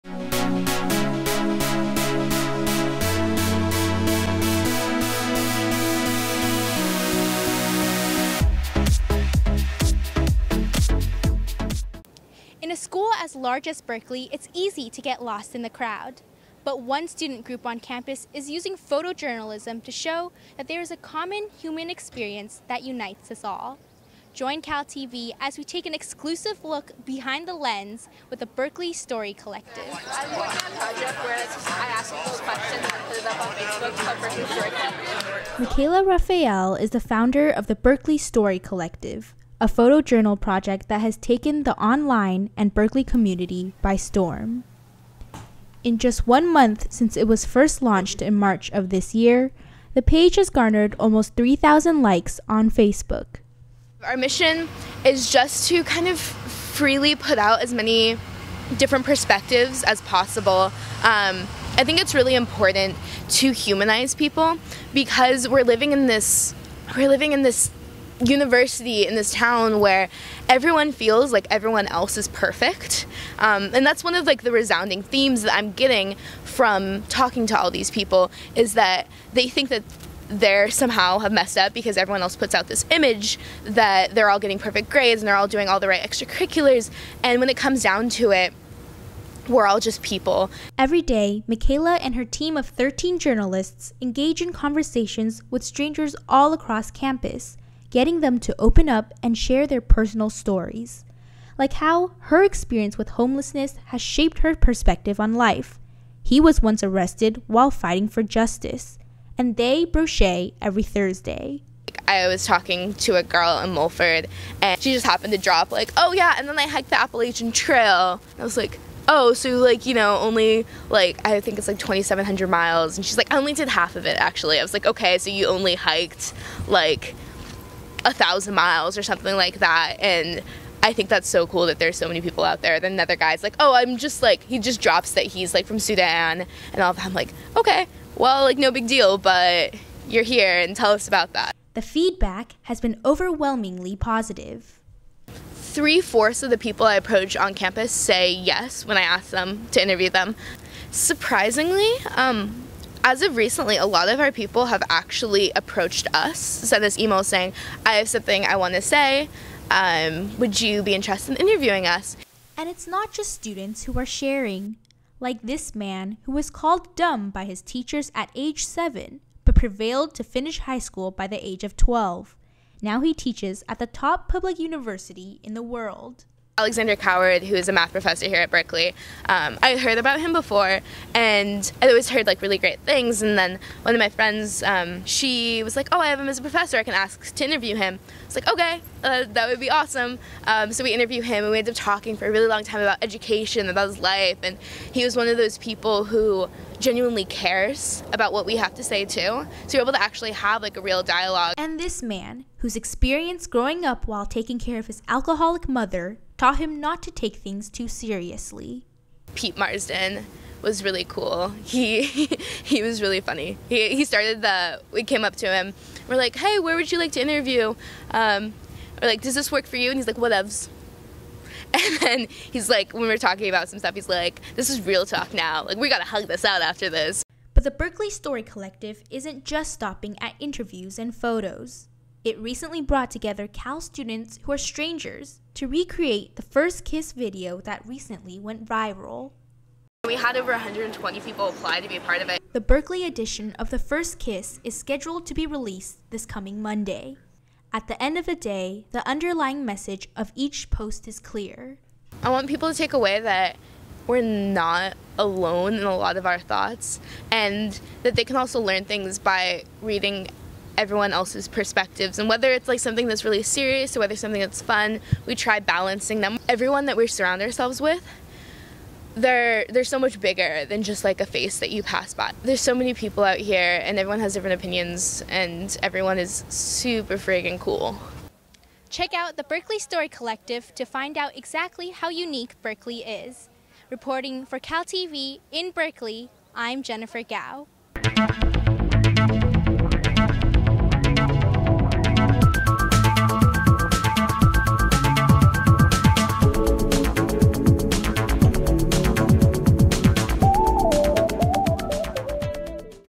CalConnect: Berkeley Story Collective
Join Jennifer Kao as she looks behind the lens of the Berkeley Story Collective.

Reported by: Jennifer Kao
Produced by: Jennifer Kao
Filmed by: Reaa Puri
Edited by: Hong (Wang) Wang, Jennifer Kao, Alvin Lin